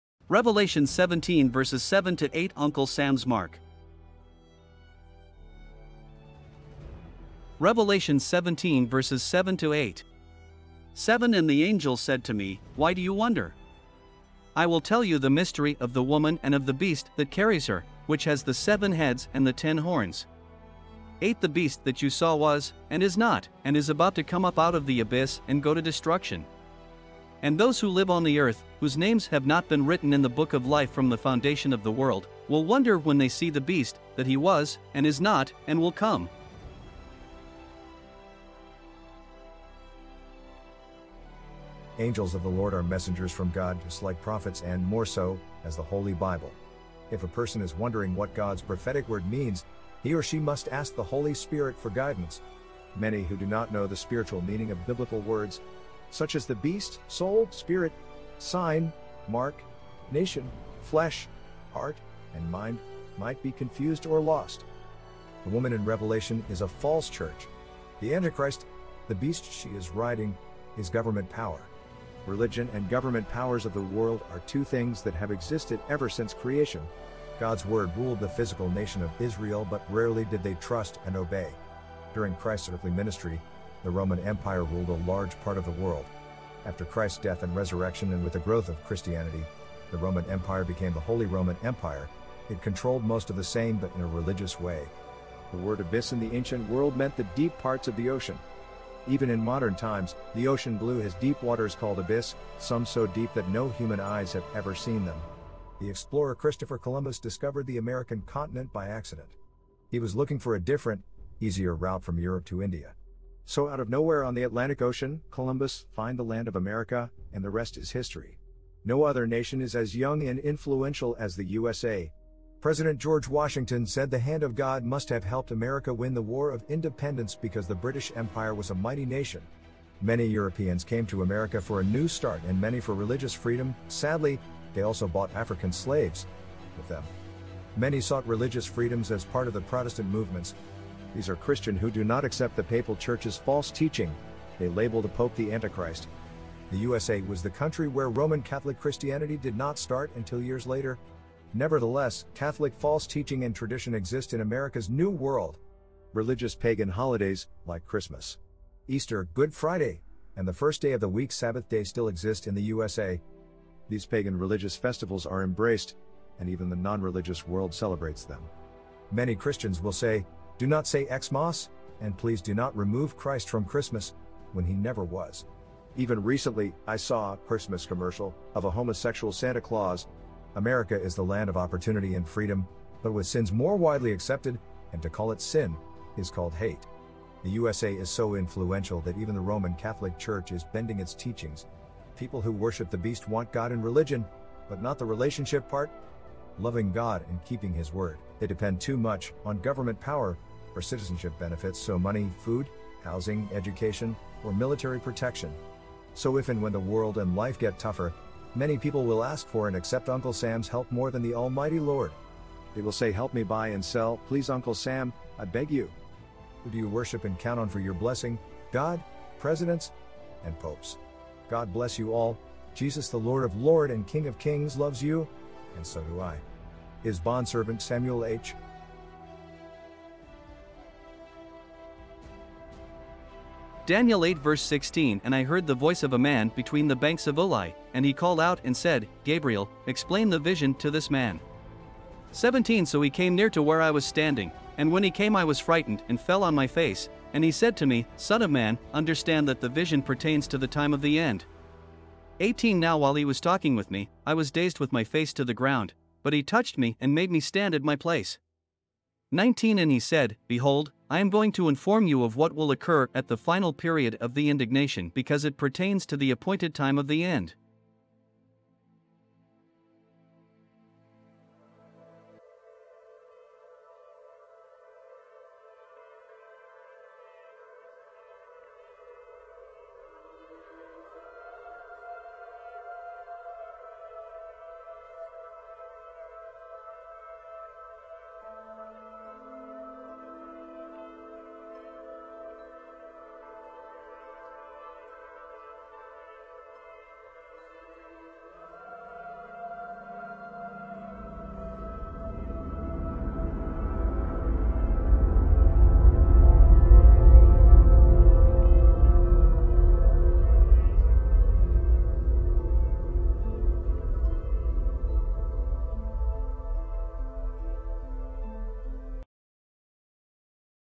Revelation 17:7-8 Uncle Sam's Mark
Revelation 17:7-8
7 And the angel said to me, "Why do you wonder? I will tell you the mystery of the woman and of the beast that carries her, which has the seven heads and the ten horns. 8 "The beast that you saw was, and is not, and is about to come up out of the abyss and go to destruction. And those who live on the earth, whose names have not been written in the book of life from the foundation of the world, will wonder when they see the beast, that he was, and is not, and will come.

Commentary Of My Personal Bible Study